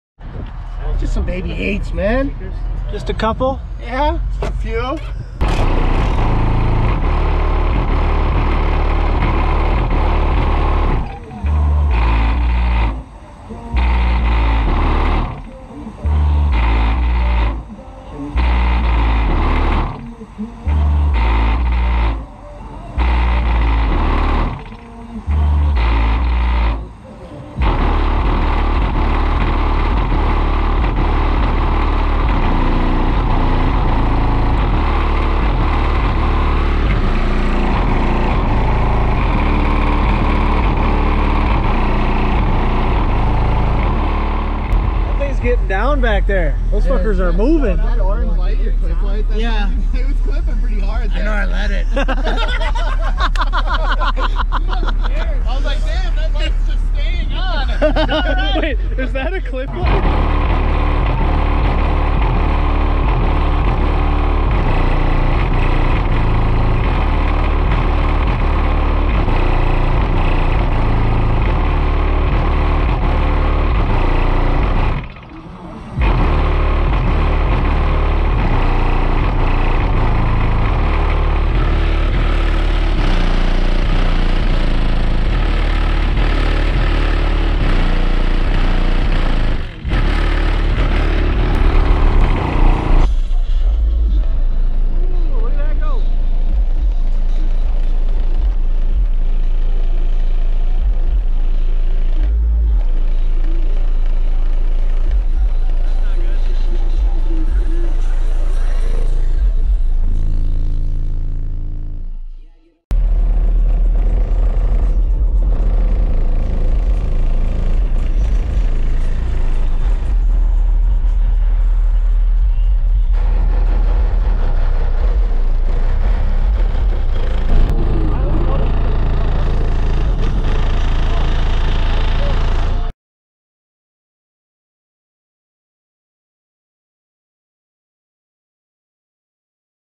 9 - 8" Subwoofers!! Sundown Audio X8s on 8000 watts
Steven's nasty nine (9) 8" subs build in his trailblazer, this thing gets DOWN.  Sundown Audio X8s on a sound digital power 8800 wired to .67 ohms !!!

Yes he knows he needs to brace his box, he just wanted it to get playing!  this video was taken at the minnesota bass heads bass meet on 3/1/2020 the spring opener!!

subwoofers purchased at directronix!